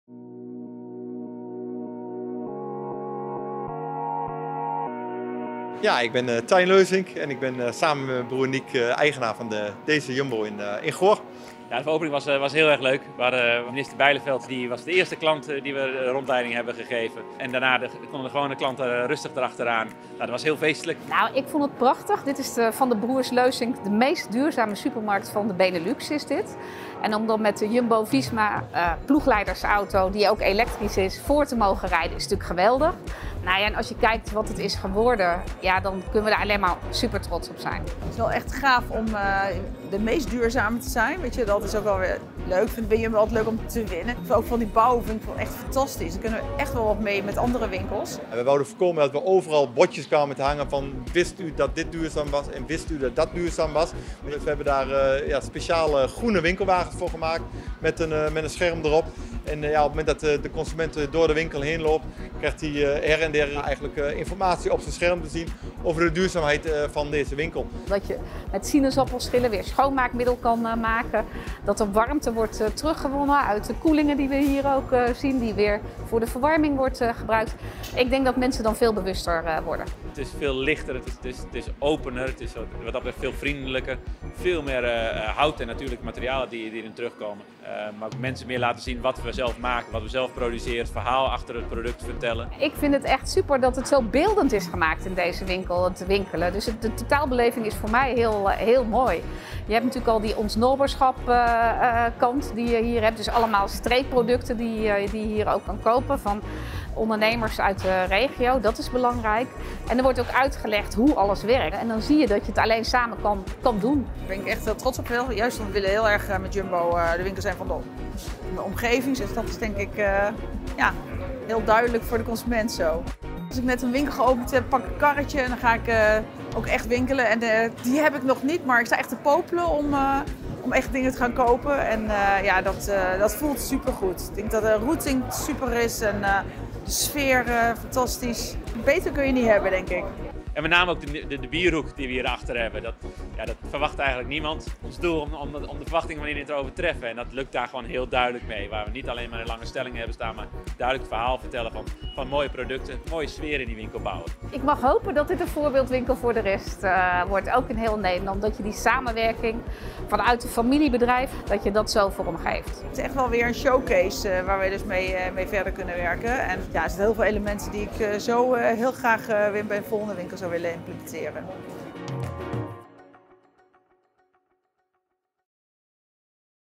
Opening Jumbo Leussink Goor
Wat een mijpalen in een dag voor UW-S bij Jumbo Leussink! Drie kidselementen met ons nieuwe karakter Pluk waaronder de nieuwe Wall met wandtablets. Minister Ank Bijleveld die met onze interactieve winkelwagens XL rijdend door de winkel uitleg krijgt van de verschillende duurzaamheidselementen van het pand. “Ons Noaberschap” als rode draad door de winkel in maatwerk behuizingen met content van ons Supermerkbureau. Colette van Eerd die haar winkelbeleving van Jumbo Goor deelt. Veel aandacht voor vers, lokaal en onderscheidend in een prachtig nieuw duurzaam gebouwd pand. Wat een mooi project en fantastisch teamwork.